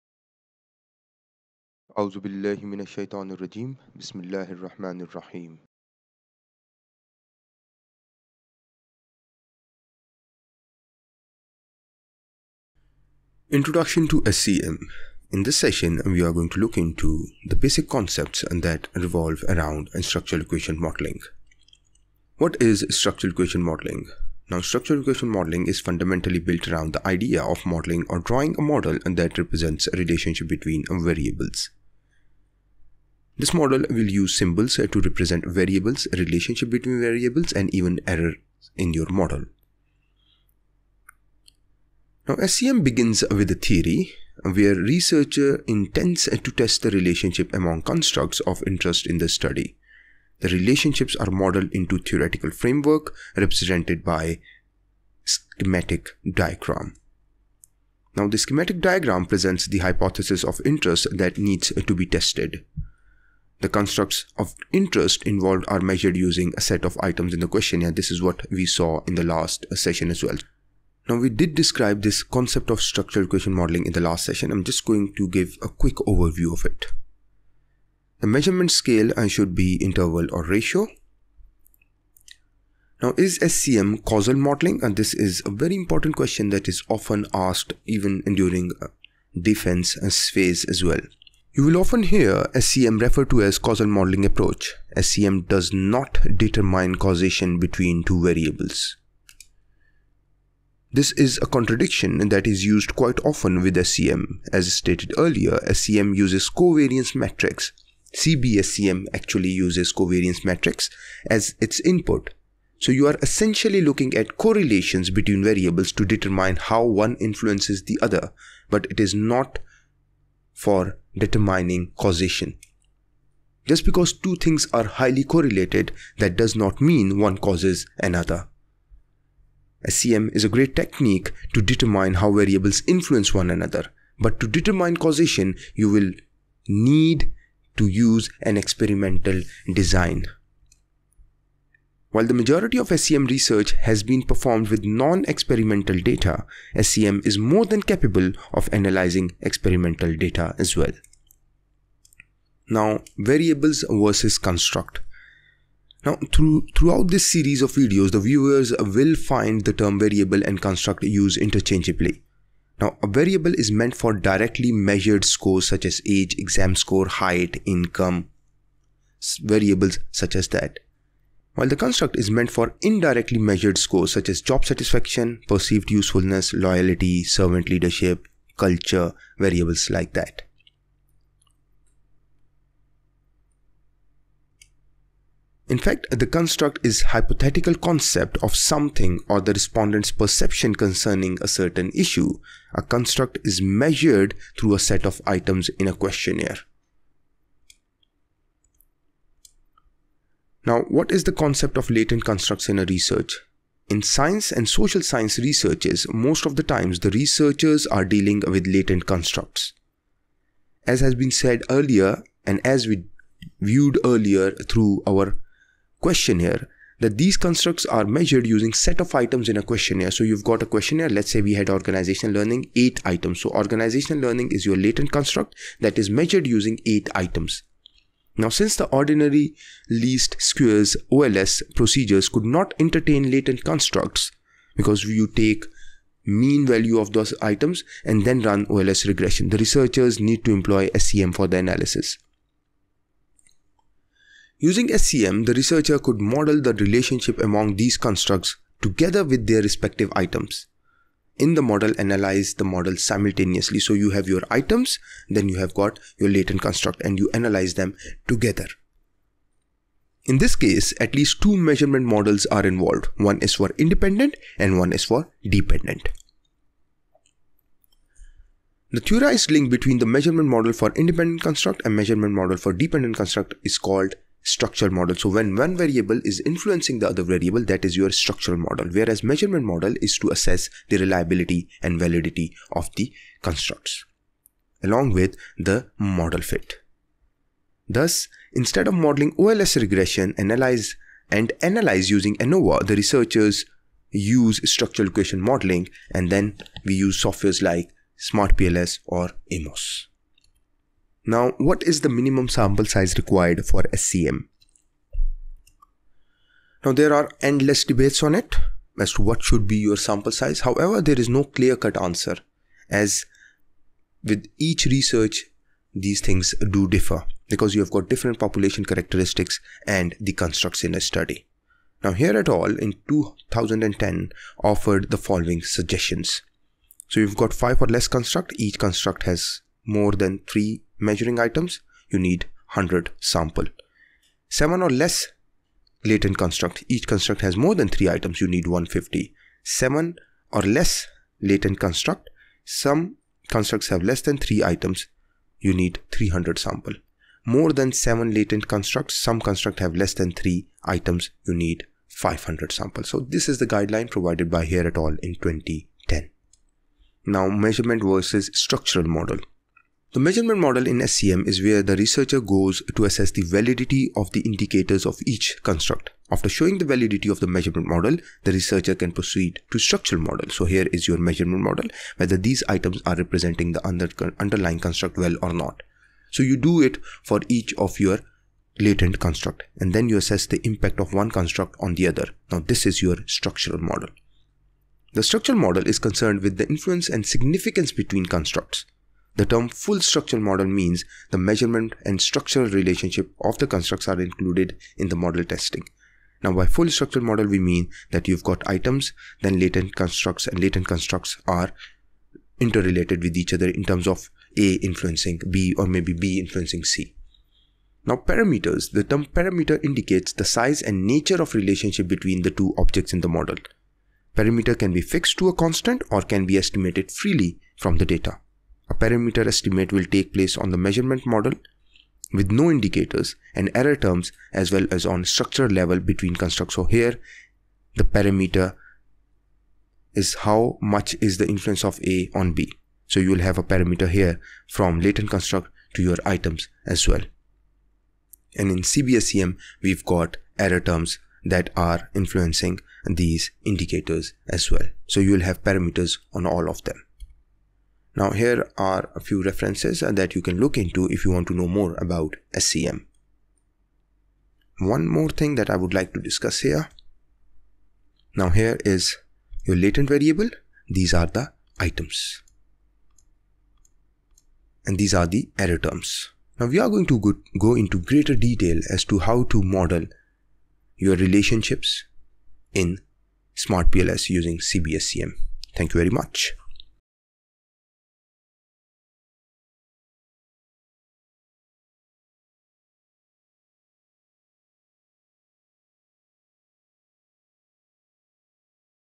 CB-SEM using SmartPLS4 - 3 - Understanding Basic Concepts in Structural Equation Modeling (SEM)
Understanding Basic Concepts in Structural Equation Modeling (SEM). In this session, we introduce the key concepts in structural equation modelling. The video focuses on the concept of structural equation modeling, understanding if SEM is causal modeling, Latent variable vs Construct and sample size in SEM.